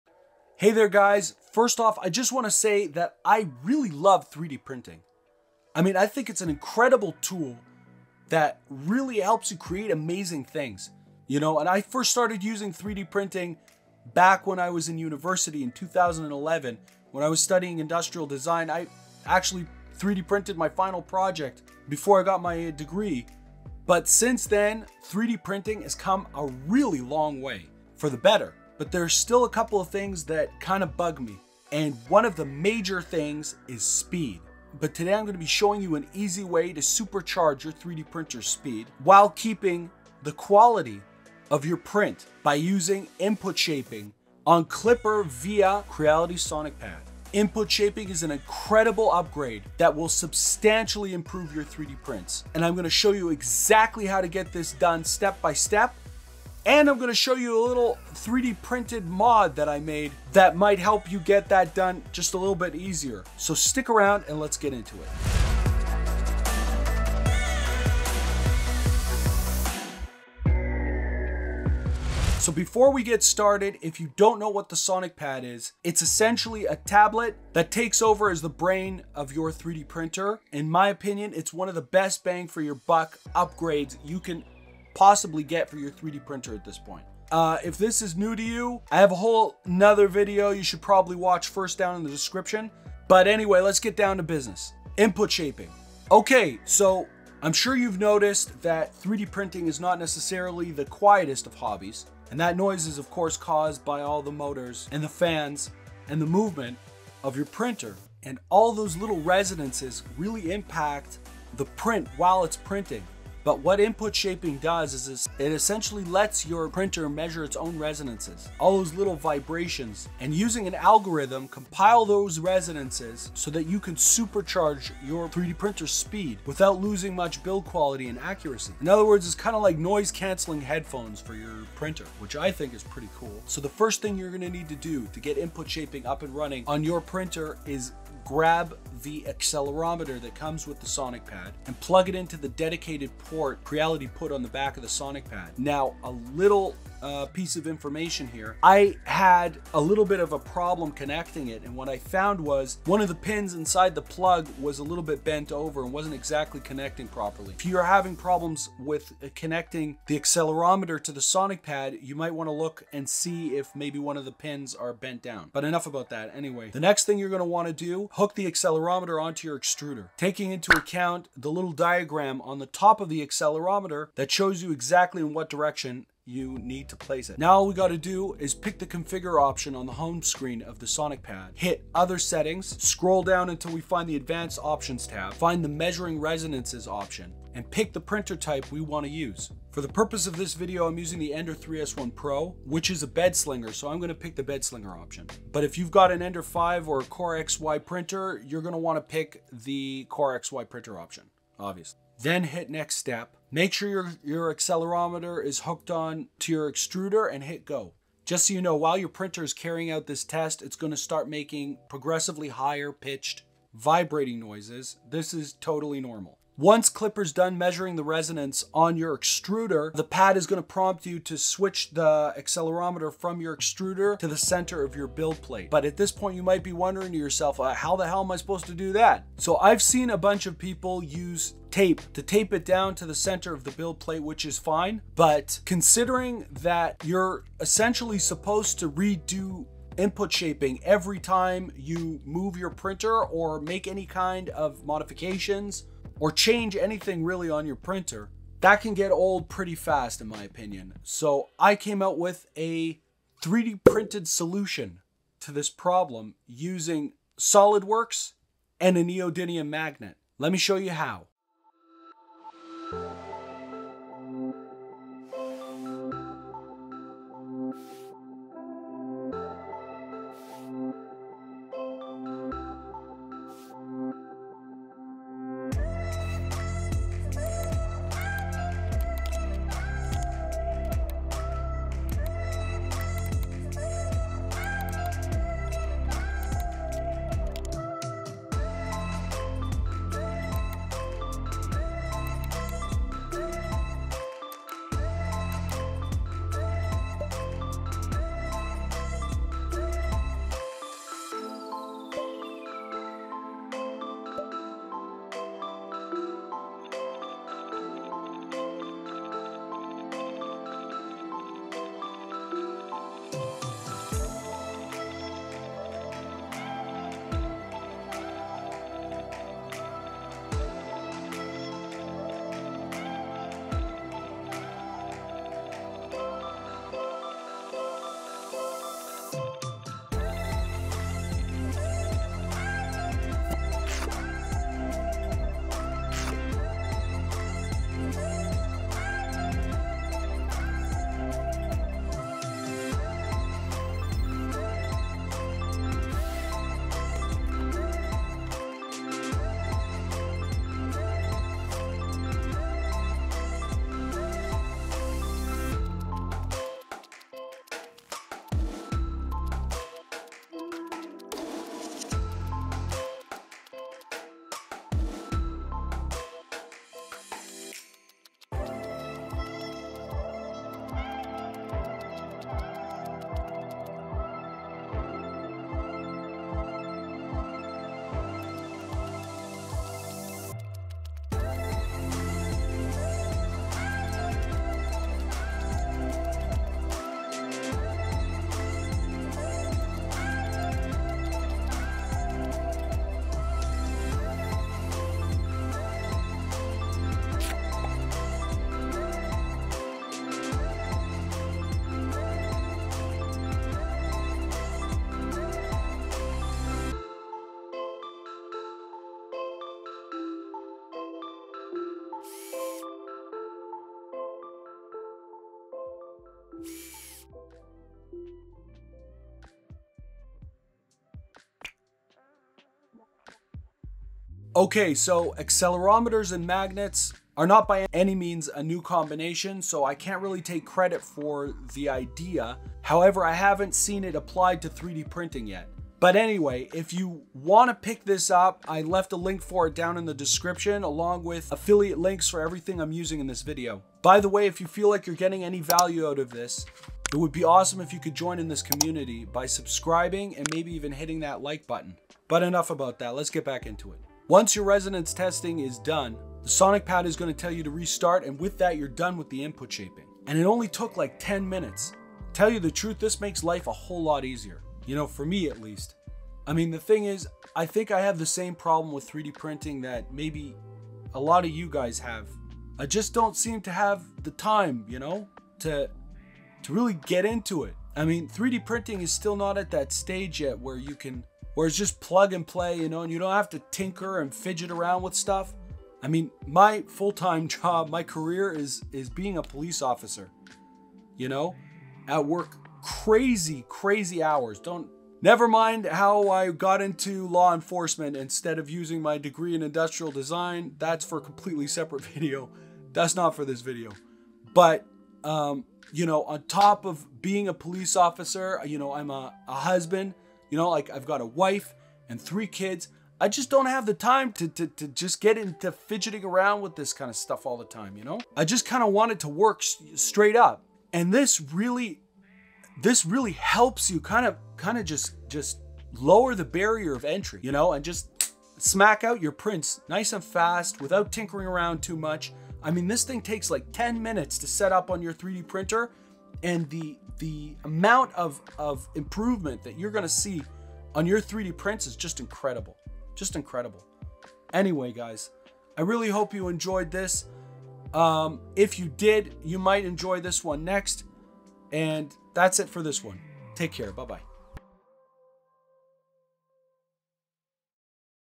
Input shaping on Crealitys Sonic Pad
Are you a 3D printing enthusiast looking to take your creations to the next level? If so, you're in luck because in this video, I'll show you how to supercharge your 3D printer's speed without compromising on quality. As someone who fell in love with 3D printing back in 2011 during my time studying industrial design in university, I know firsthand the incredible potential this technology holds. However, there are still a few things that can hold us back, and one of those is print speed. But fear not, because I've found an incredible upgrade that will change the game for your 3D printing projects. By using input shaping on Klipper via the Sonic Pad, you can achieve lightning-fast print speeds without sacrificing quality. And I'll guide you step-by-step through the process, so you can implement this game-changing upgrade with ease. Plus, I'll even share a 3D printed mod I made that will make the process even smoother. Trust me, you won't want to miss this tutorial. So, join me and let's take your 3D printing to the next level

Link to the sonic pad on Amazon
Link for some JB weld epoxy
Link for the printer i use
Mitutoyo digital caliper

Link to my Etsy store